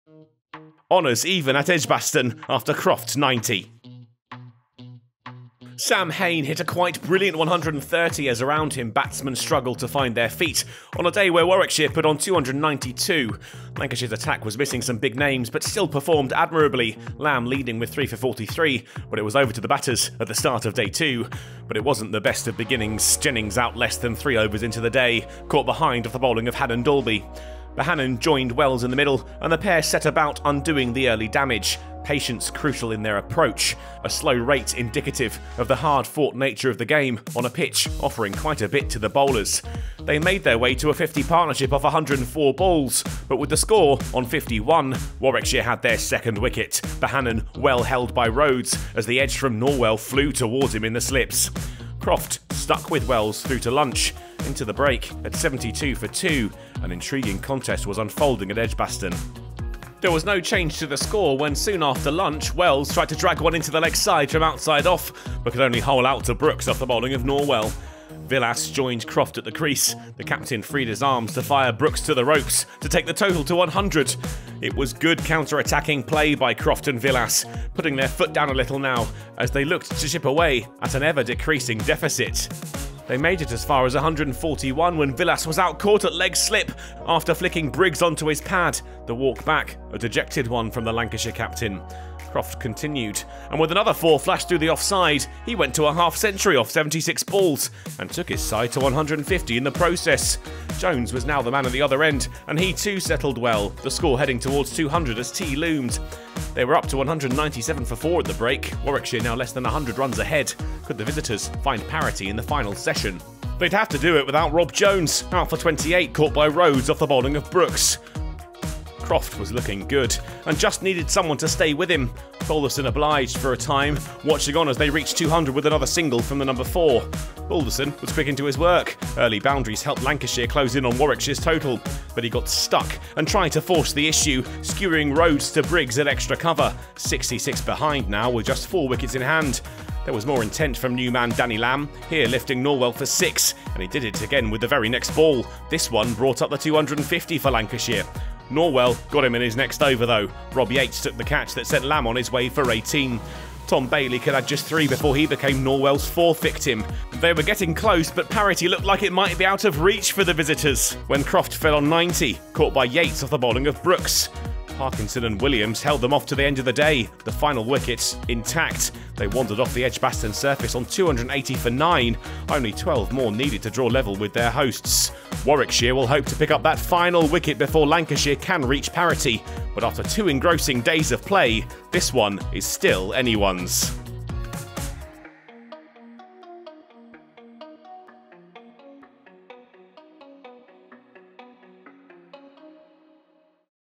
HIGHLIGHTS | Warwickshire v Lancashire | County Championship | Day Two
The second day of an absorbing LV= Insurance County Championship match at Edgbaston ended with the game fine balanced.

On an intensely competitive, hard-fought day, Lancashire’s first innings was a mirror image of Warwickshire’s the day before. Where Sam Hain’s century had underpinned the home side’s batting with only sporadic support, Croft formed a similar backbone for the Red Rose with similarly bit-part contributions from his colleagues.

Liam Norwell, playing his first game back after injury, led the Bears’ bowling with four for 78.